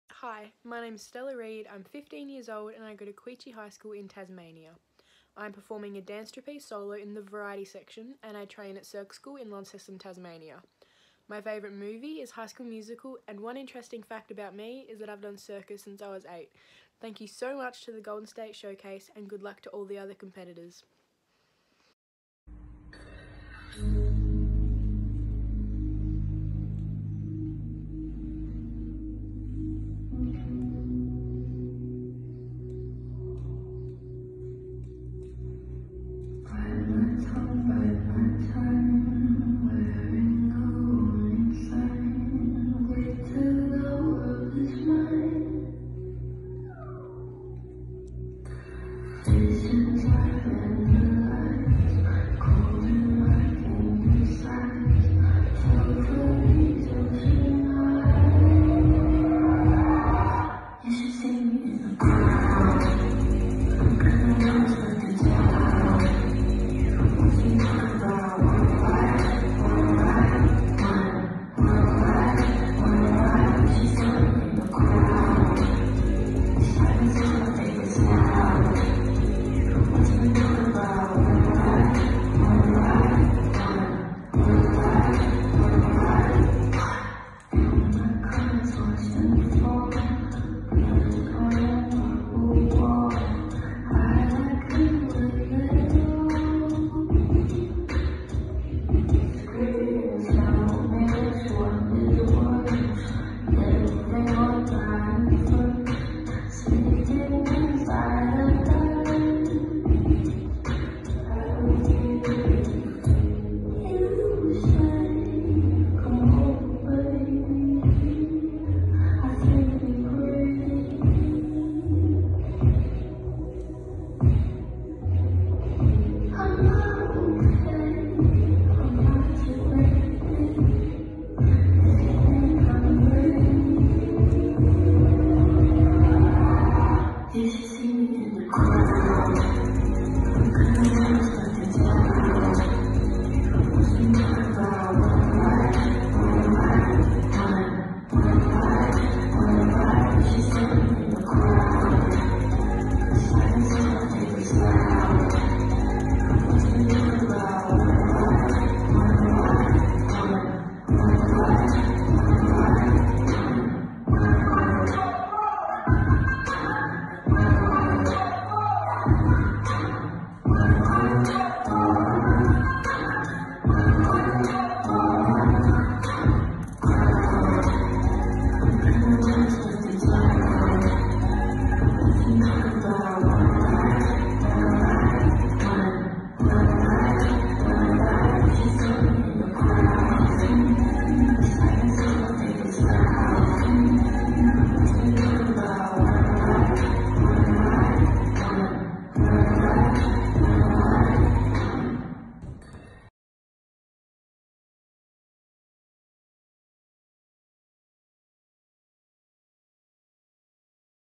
Golden State Showcase September - Stella Reid (Variety)
Golden State Showcase September Qualifier Stella Reid  performs for the 13-18 Variety Solo Category.

Golden State Showcase does not own the rights to this music. All use of this material is for educational and entertainment purposes only.